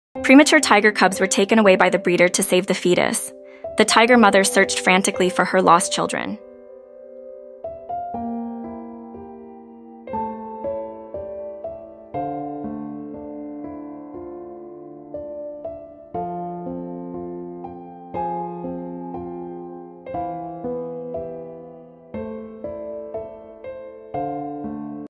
Premature tiger cubs were taken away by the breeder to save the fetus
Premature tiger cubs were taken away by the breeder to save the fetus. The tiger mother searched frantically for her lost children